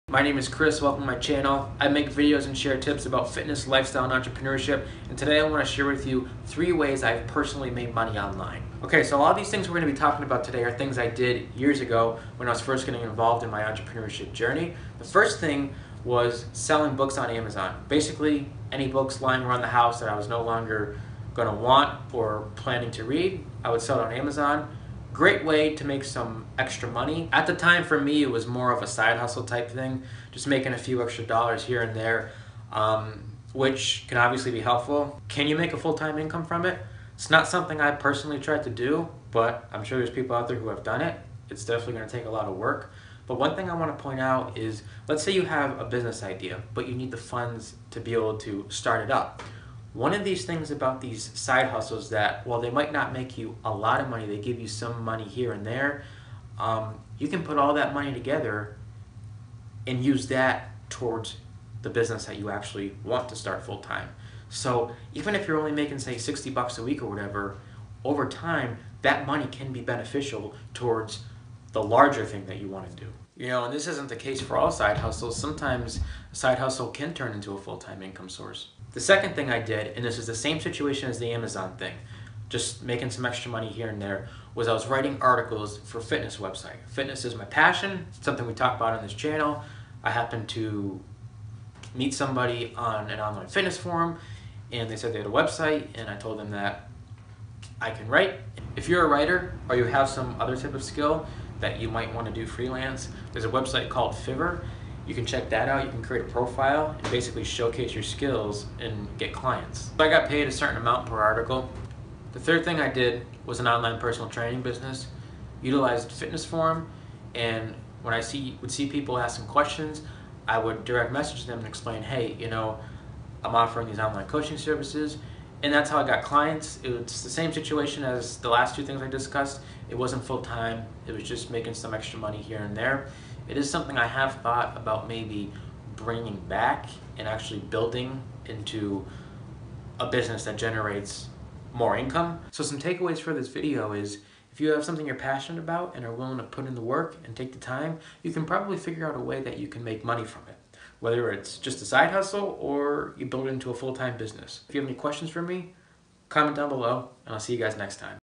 I Started 3 Side Hustles to Make Money From Home | How to Make Money Online
Intro: 0:00
#1 Amazon Side Hustle: 0:15
#2 Writing Side Hustle: 1:19
#3 Coaching Side Hustle: 1:52

Hey there! My name is Chris and I make videos about Fitness, Lifestyle, and Entrepreneurship. In this video I am discussing three side hustles I started to make money online from home, with advice and tips for your side hustle / business.